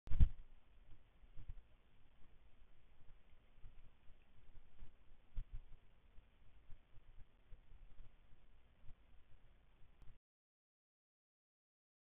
Meteor/fireball Conway South Carolina 12/3/2021
Wyze camera caught a meteor/fireball 1:11 am 12/3/21 Conway South Carolina